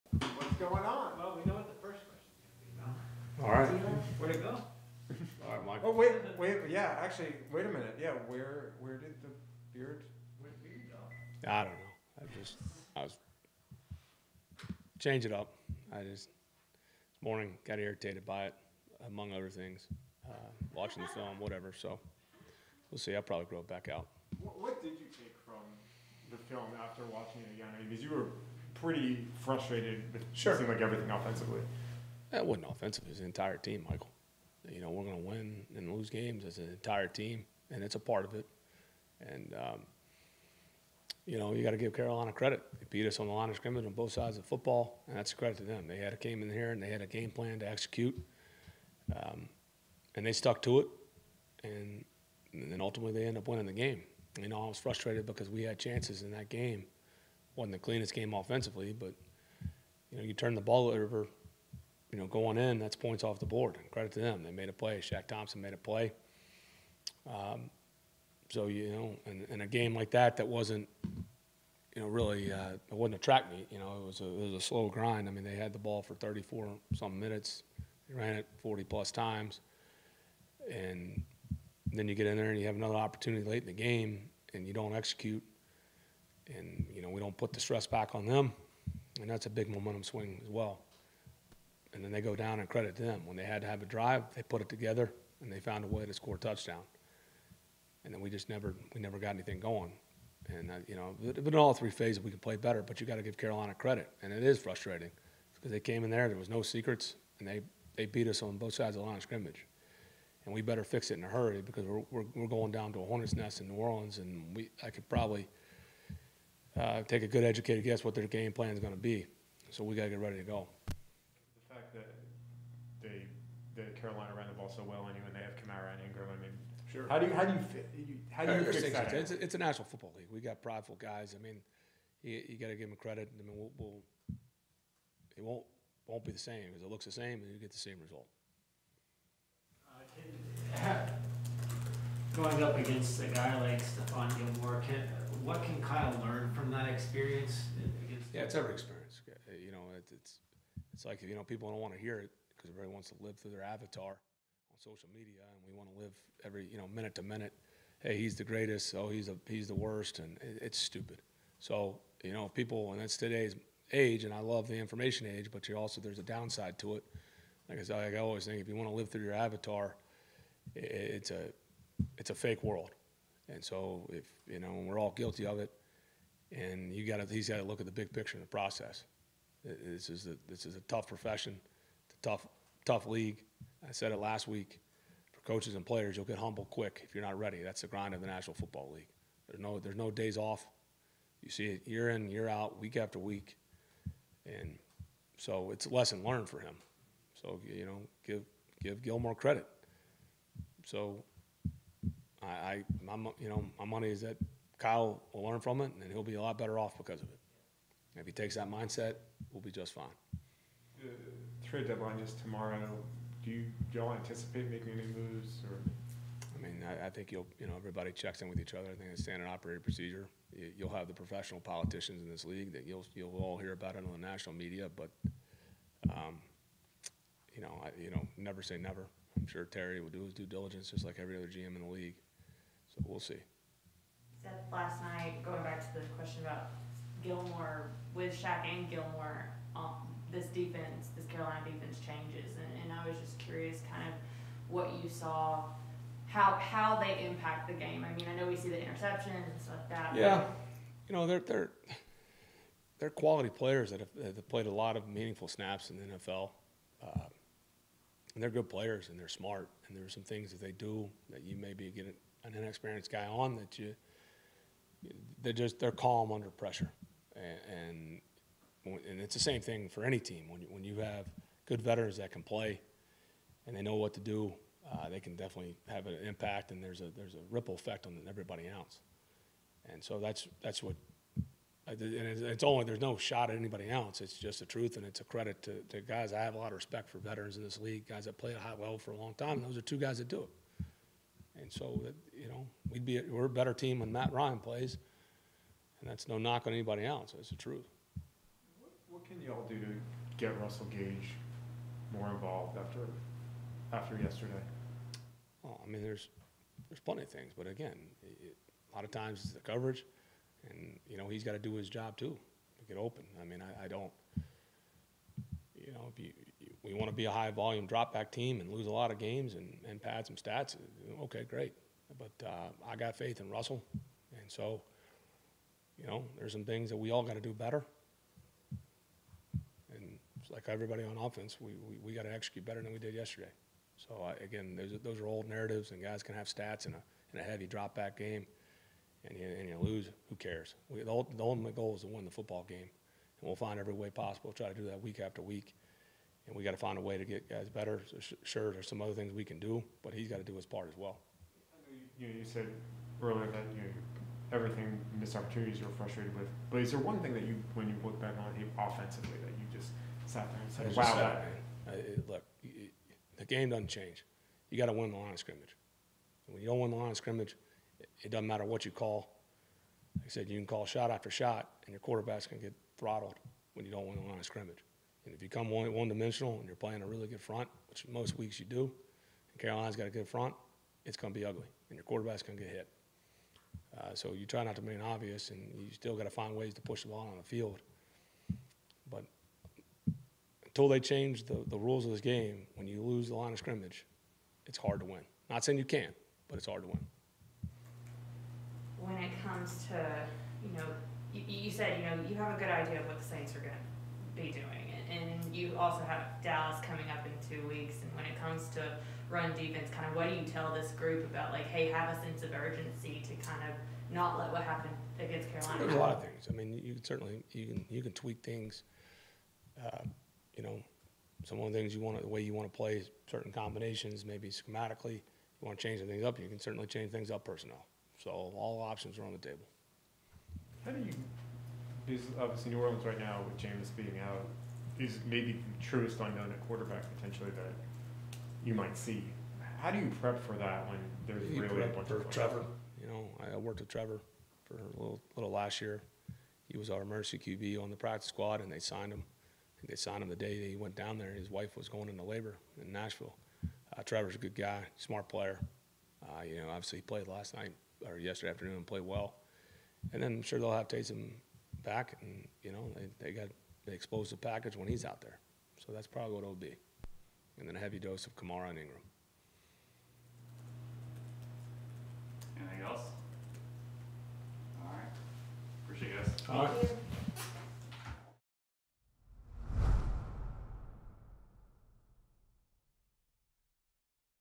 'When you lose the line of scrimmage its hard to win' | Arthur Smith | Atlanta Falcons | NFL
Arthur Smith speaks on the upcoming trade deadline, Carolina's defense, and how the team has to get ready to travel to New Orleans for their matchup against the Saints.